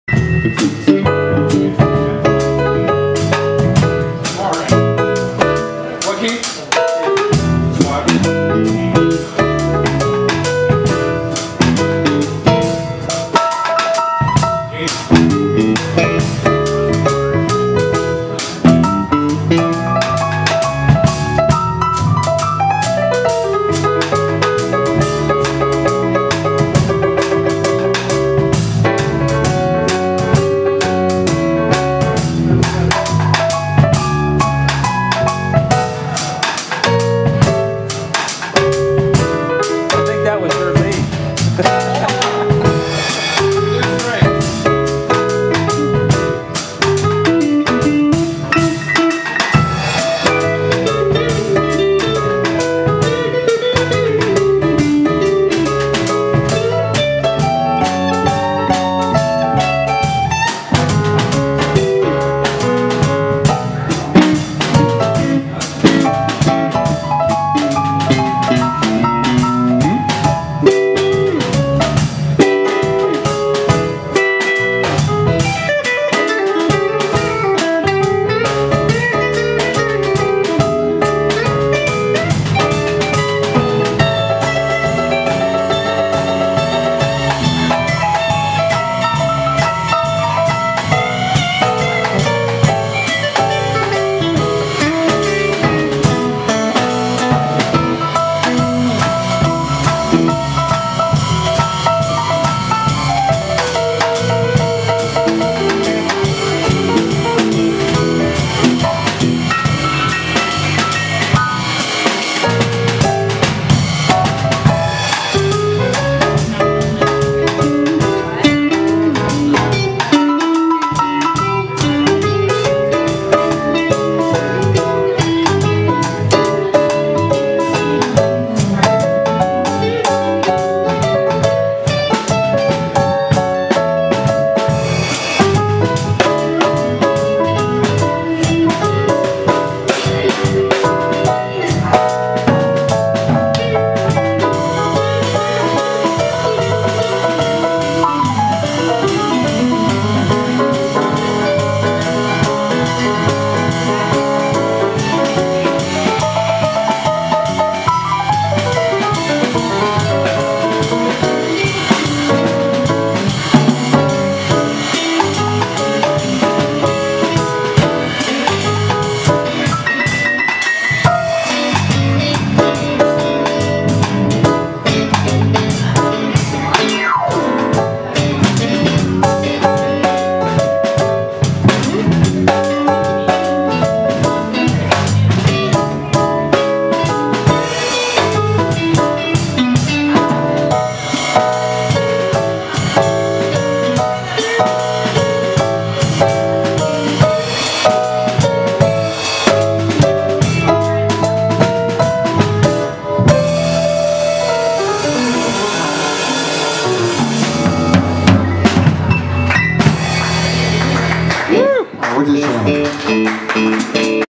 Opening Jam...fun tune..
Gracie Rae's Blues Jam...Steve Wiggins host on the keys, 12 Gage Blevins on guitar, Tim Carson on the drums. nice